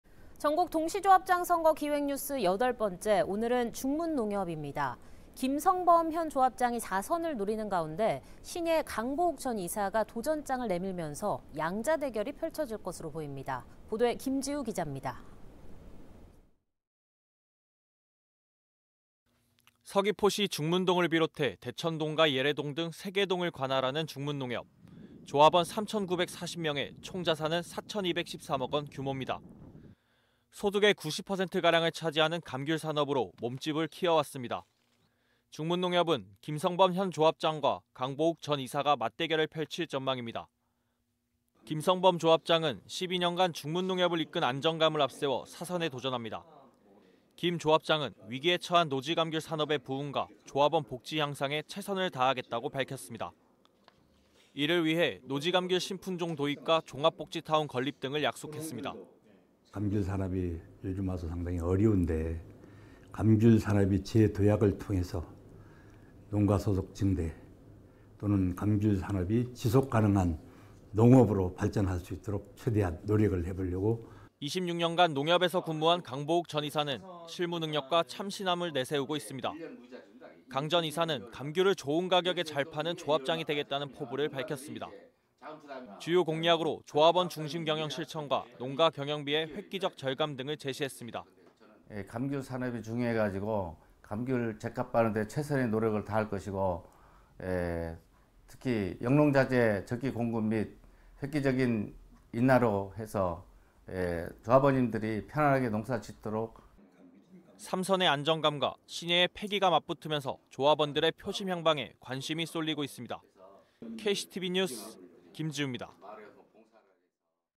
[조합장선거 누가 뛰나 8] 중문농협…‘3선’ vs ‘신예
전국동시조합장선거 기획뉴스 여덟 번째, 오늘은 중문농협입니다.

김성범 현 조합장이 4선을 노리는 가운데 '신예'의 강보욱 전 이사가 도전장을 내밀면서 양자 대결이 펼쳐질 것으로 보입니다.